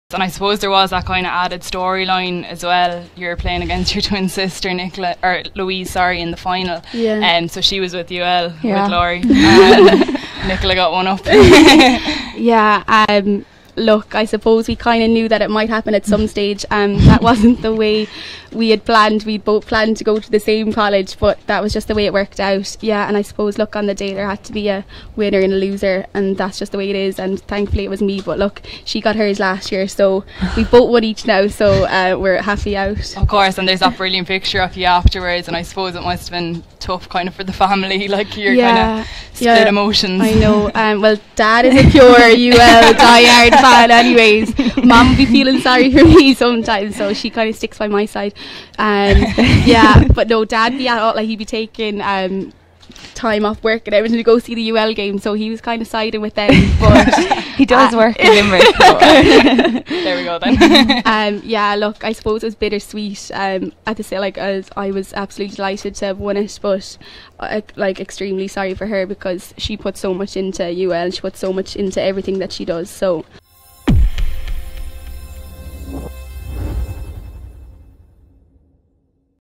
'On the day, there had to be a winner and a loser'
In 2016, Nicola Ward scored the crucial goal for UCD as they were crowned Gourmet Food Parlour O'Connor Cup champions for the first time in ten years.

On that occasion, Nicola lined out against twin sister Louise, who was in the colours of UL.

Another meeting could be on the cards again this weekend as Nicola, the UCD captain, and Louise find themselves on opposite sides of the semi-final draw.

Speaking on the Ladies Football show, Galway star Nicola looked back on two years ago, and discussed a possible repeat showdown...